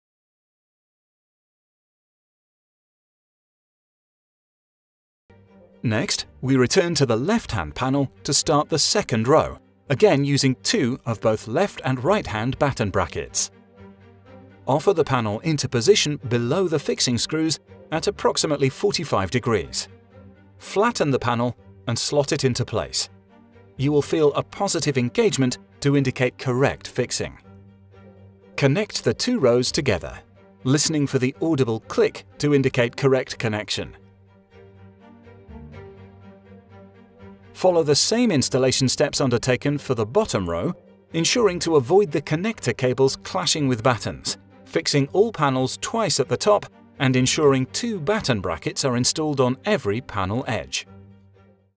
Second Row Panel Installation | In-Roof Solar Installation Part 10
Learn how to install Sandtoft in-roof solar. Part 10 covers how to install the second row of solar panels.